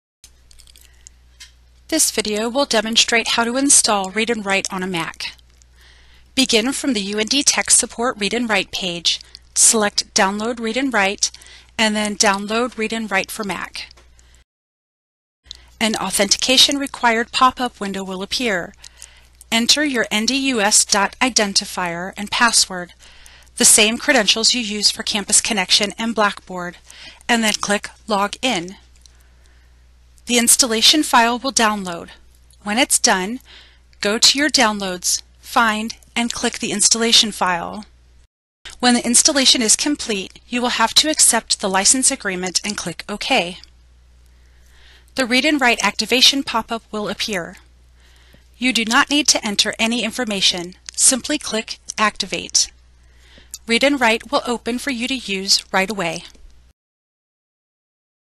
Read&Write: Mac Installation
This video will demonstrate how to install Read&Write on a Mac computer.